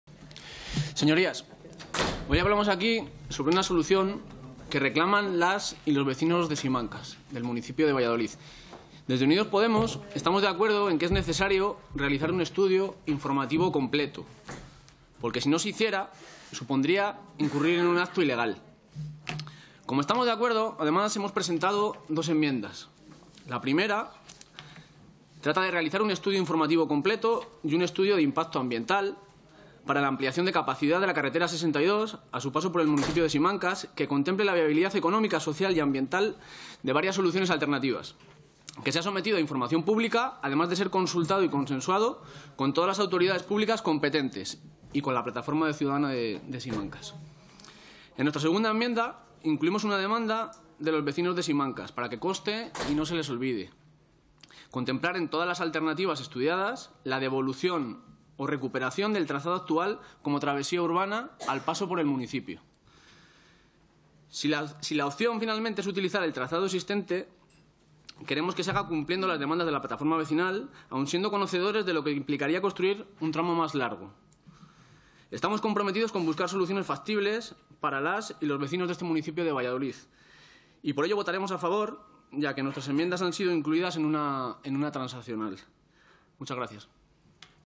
Juanma del Olmo, diputado de Podemos, defiende la solución de la Plataforma Humanizar Simancas
En la Comisión de Fomento el 29 de Noviembre